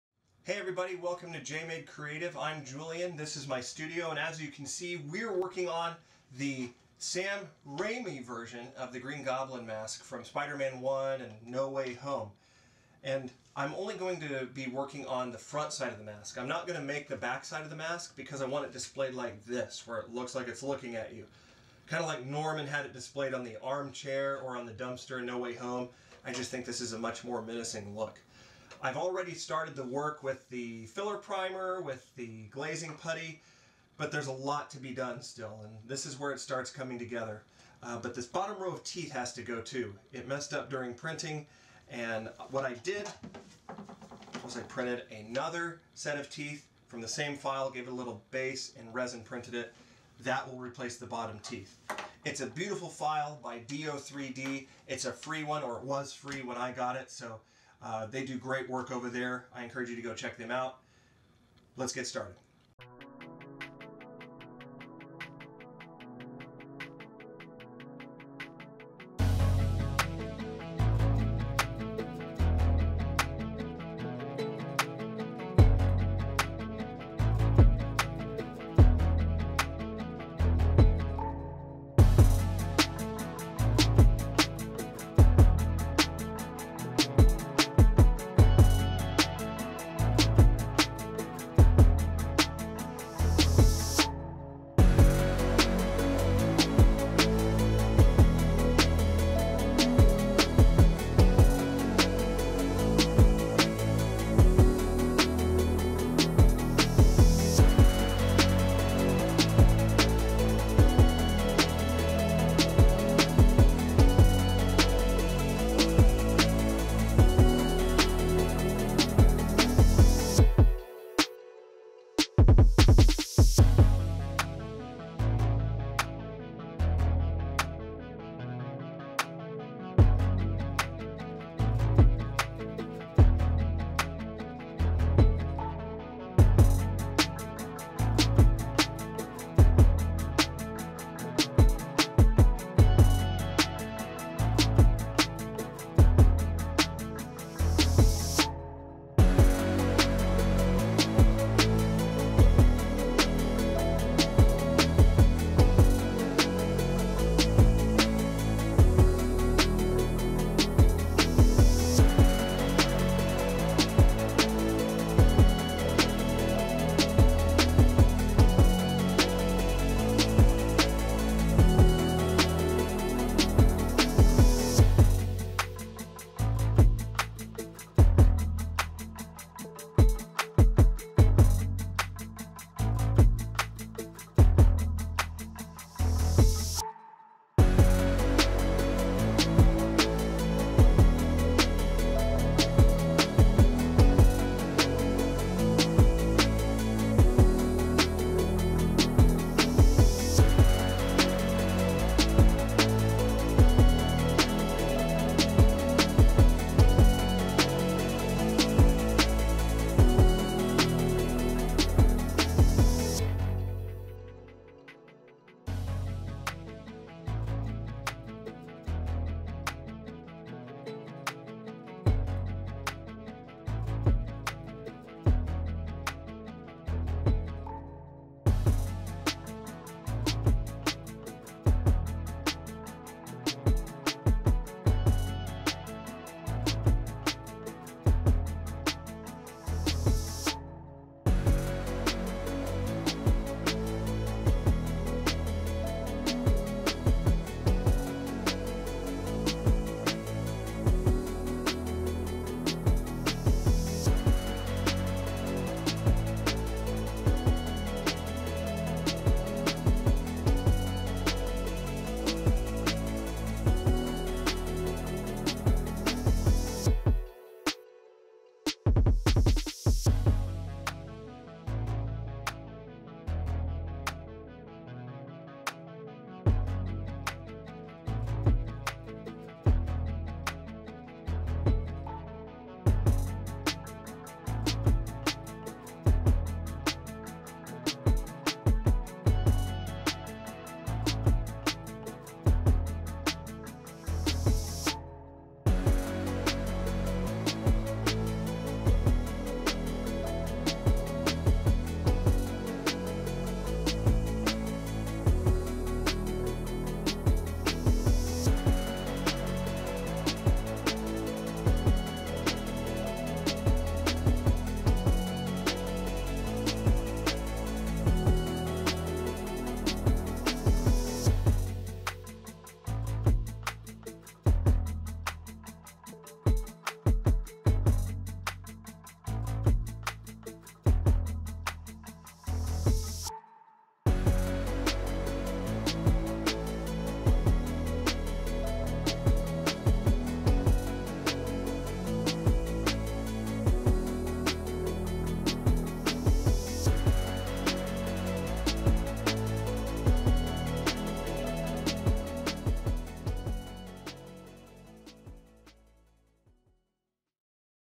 How to finish a 3d printed Green Goblin mask for cosplay
In this video, Julian shows the continuing build progress on a 3D printed, PLA, Marvel studios version of Green Goblin's mask made from a file ￼downloaded from do3D.com.￼

This mask is great for cosplay.

As the penultimate ￼villain of Spider-Man, Green Goblin's mask strikes fear from the frontal view.

In the timelapse, Julian sands down the seams , removes the bottom row of teeth with a flex shaft tool, ￼and replaces them with a 3-D printed resin version,  and finally glues it down with JB Weld epoxy.￼